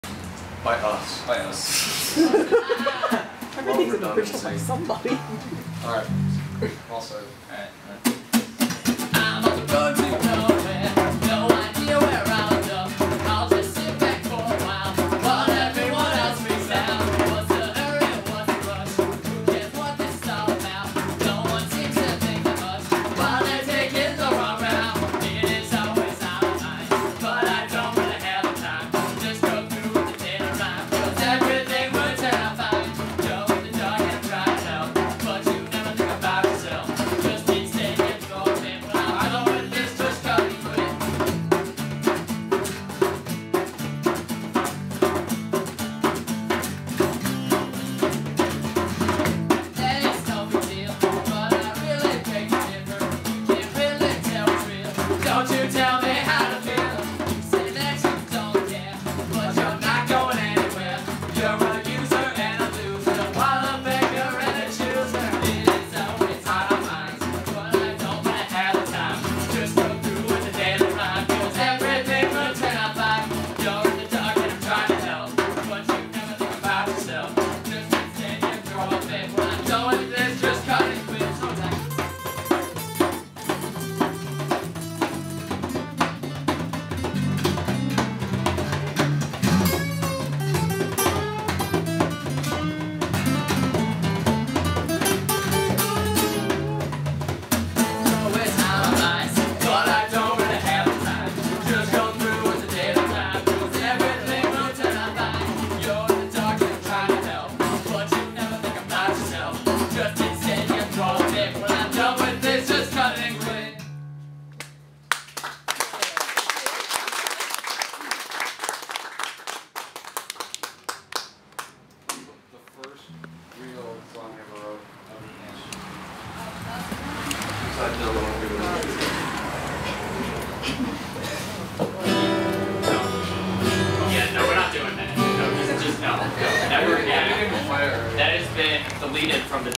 The Blind Vines
Playing at Rediscover Records in Elgin, IL